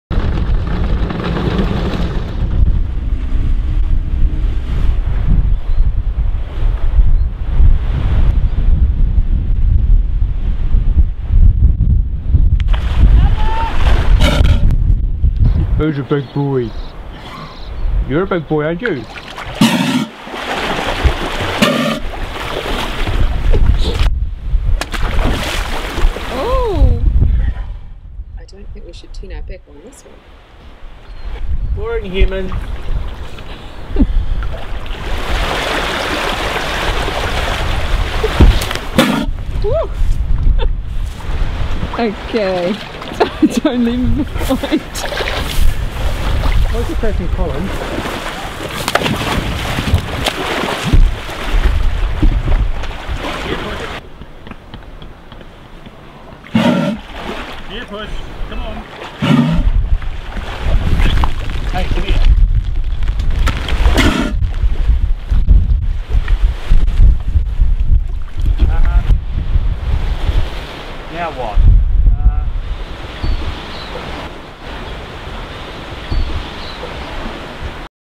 A Sea Lion with Brawn
Our return trip to Northeast Stream was somewhat more 'exciting' than that of a few weeks ago. This time we encountered a large male sea lion that had a bee in his bonnet. All I have to say is "sometimes brawn wins over brain".